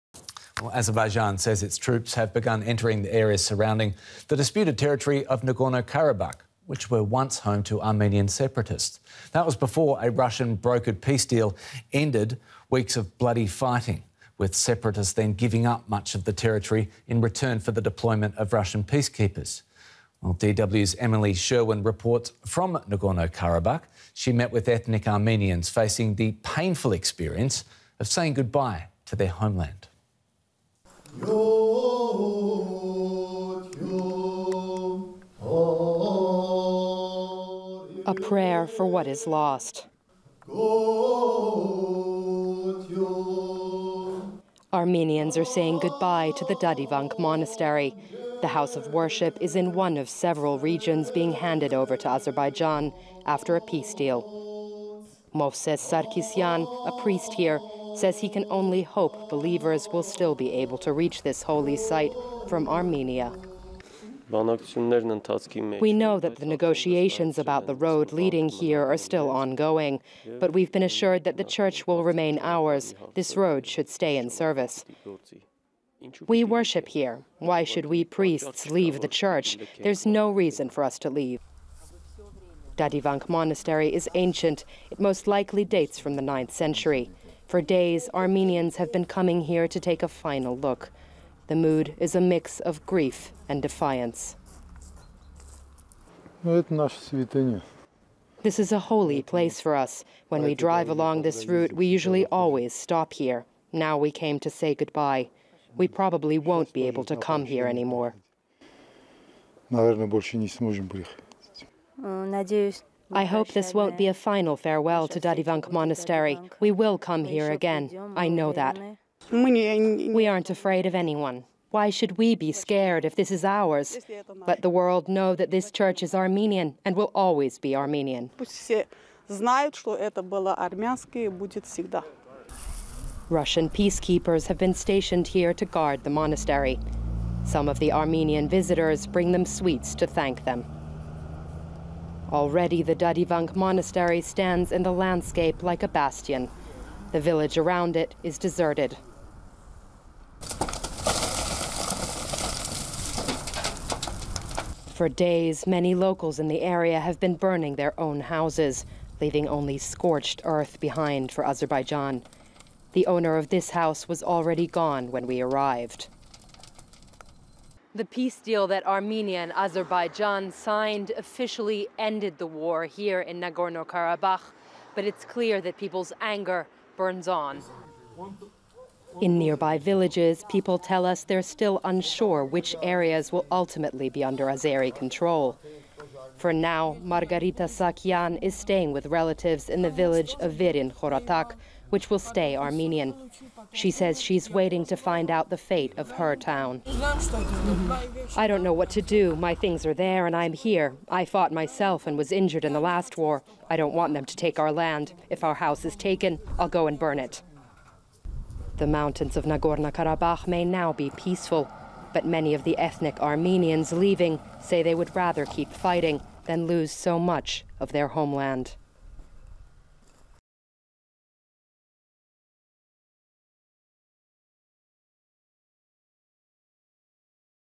Nagorno-Karabakh: Armenians say goodbye as Azeri troops move in | DW News
Azerbaijan says its troops have begun entering areas surrounding the disputed territory of Nagorno Karabakh, which were once home to Armenian separatists. That was before a Russian-brokered peace deal ended weeks of bloody fighting, with separatists then giving up much of the territory in return for the deployment of Russian peacekeepers. DW's Emily Sherman reports from Nagorno Karabakh. She met with ethnic Armenians in Kalbajar facing the painful experience of saying goodbye to their homeland and its iconic Dadivank Monastery.